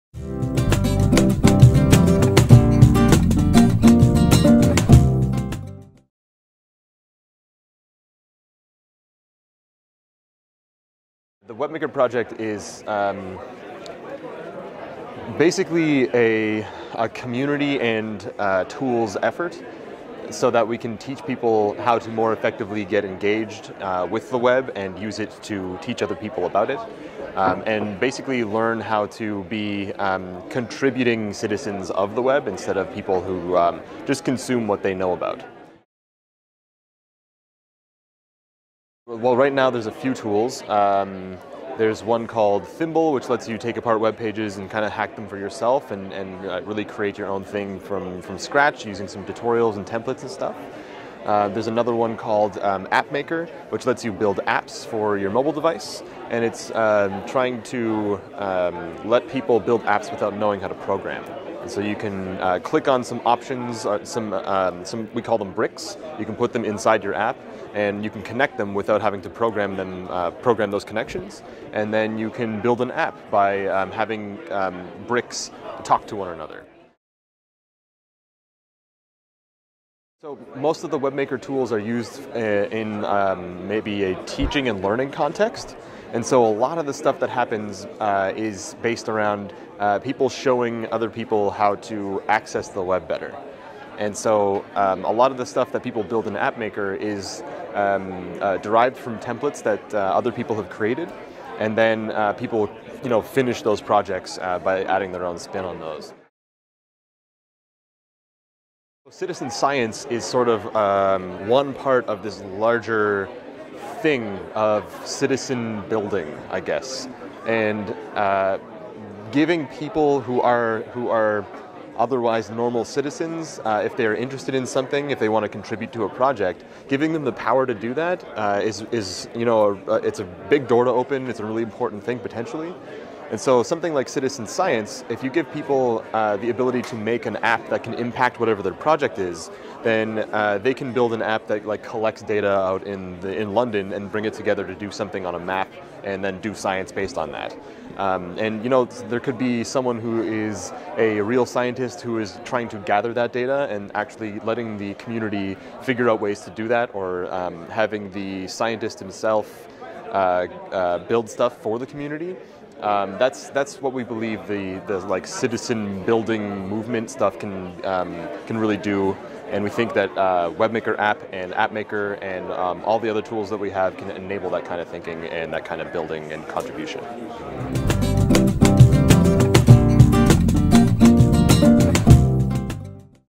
Mozilla Appmaker
We met up with Bobby Richter at Mozfest 2014 ) to talk about AppMaker )—Mozilla’s new platform for “codeless” mobile app creation .  Bobby Richter is a developer and producer at Appmaker

Bobby was interviewed by Julie Gould, who also produced the video, for the Mobile Collective on behalf of the Citizen Cyberlab Project